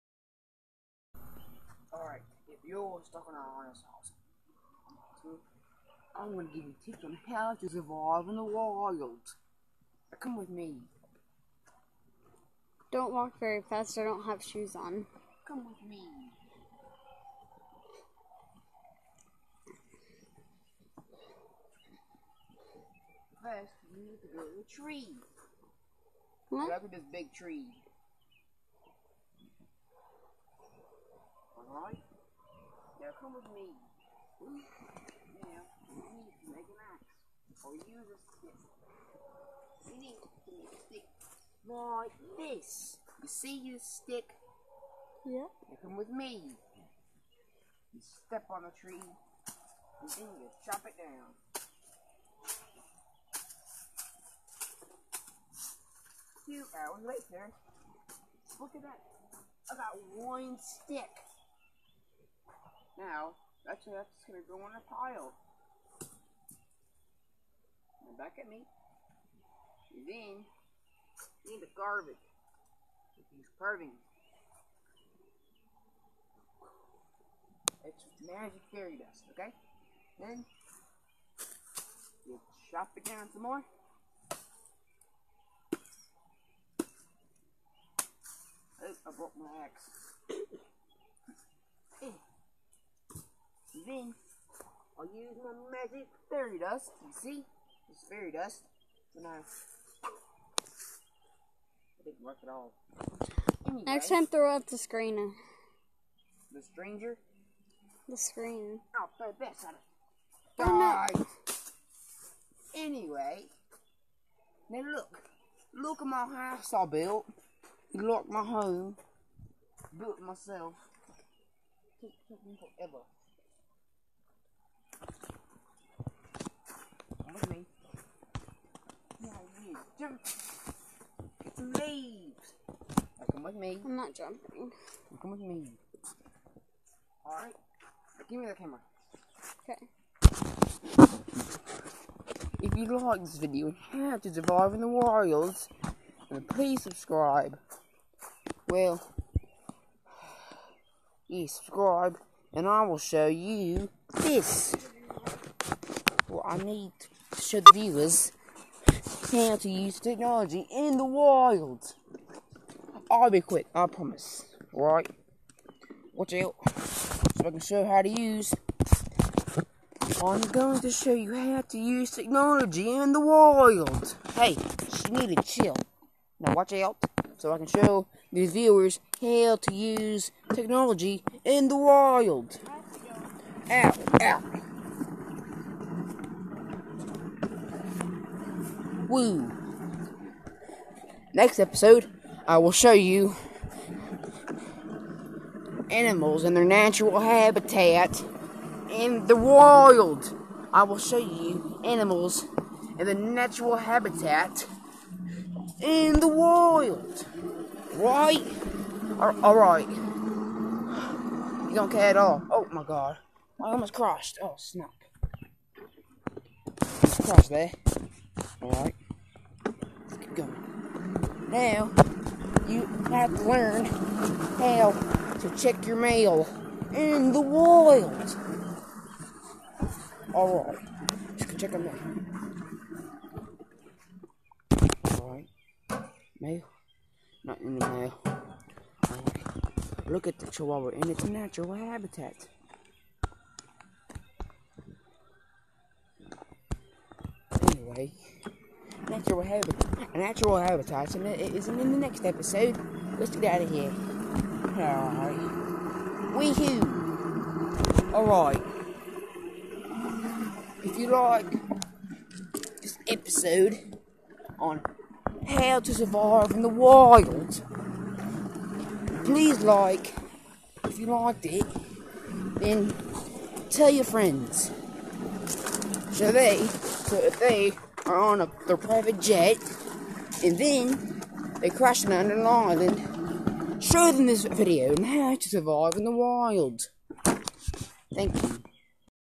How to survive in the wild
If your private jet crash lands on an island
Watch this video so you don't get eaten by animals
And if your friends get into the same problem
Show them this video NOW!